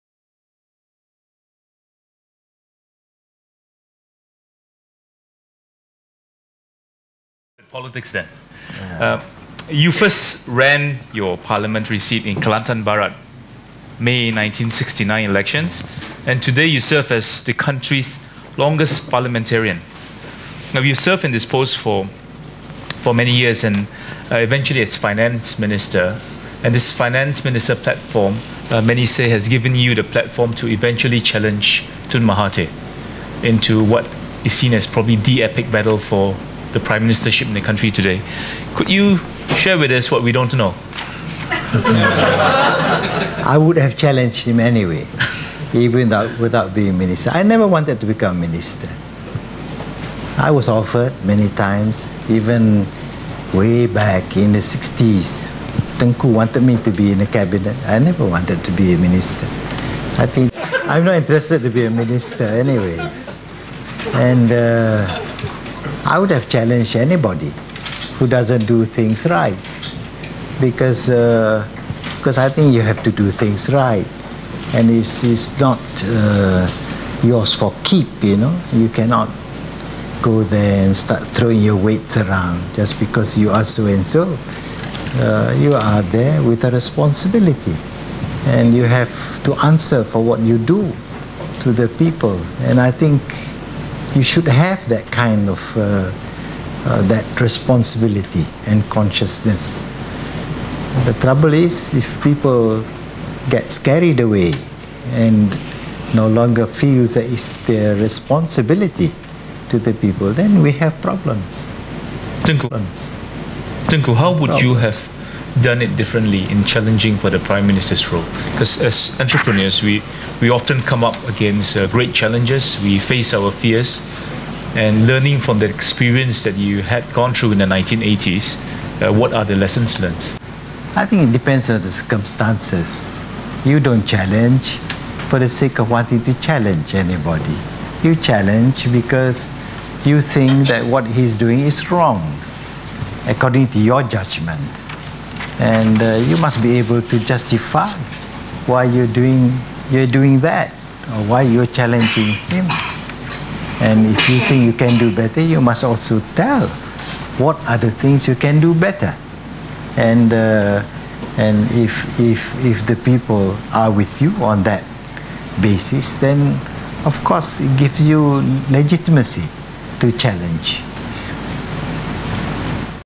Tengku Razaleigh 2012 - Why I challenged Mahathir?
Tengku Razaleigh sharing some of his experience and thoughts in politics, why he challenged Mahathir, ministerial posts with the audience in a gathering with young entrepreneurs in KL